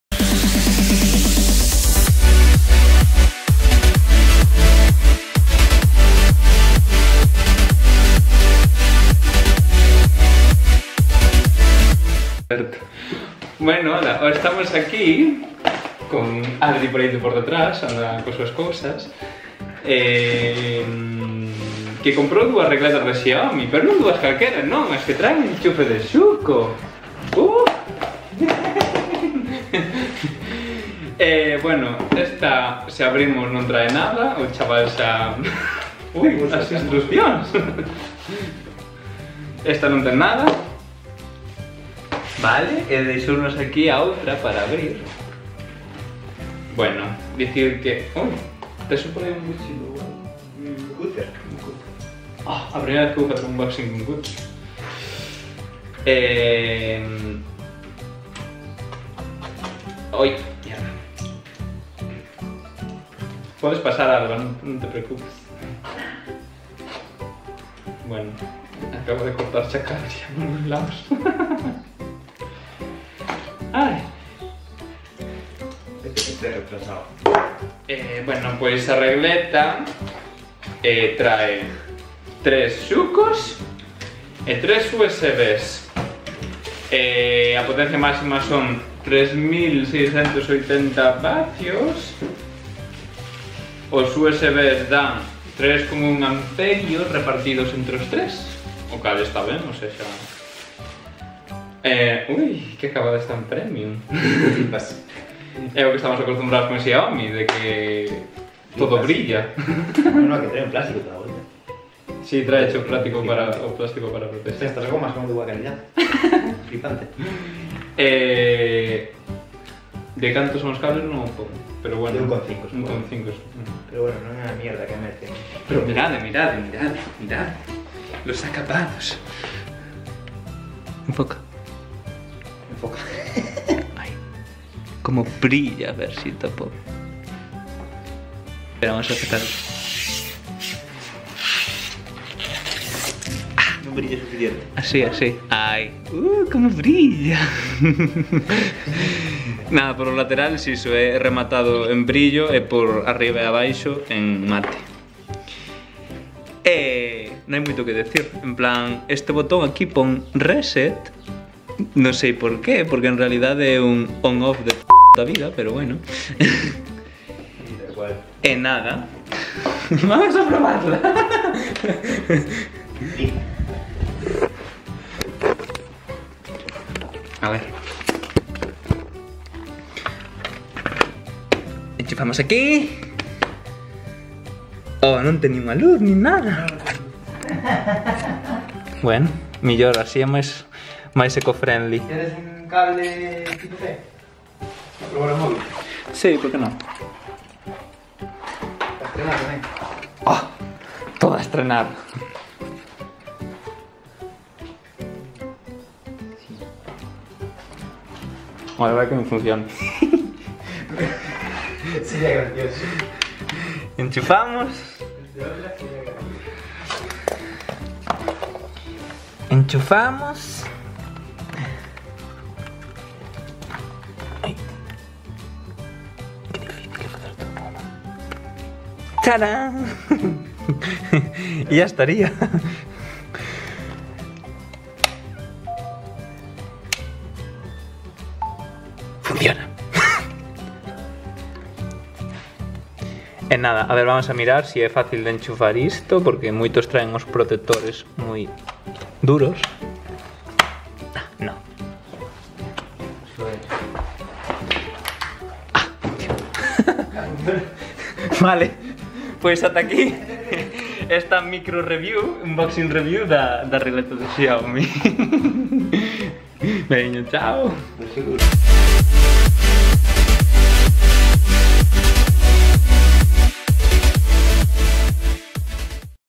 Regleta Xiaomi - Versión Schuko
Moi boas! Hoxe ensínovos rapidísimamente un producto (a verdade pouco tecnolóxico) de Xiaomi. Para que vexades a cantidade de produtos que fabrica esta xente!
Trátase dunha regleta de 3 Schukos e 3 USB's, nada máis. Pero que a diferenza do resto de regletas ten un prezo competitivo e ofrece uns acabados moi bos!

Link de compra: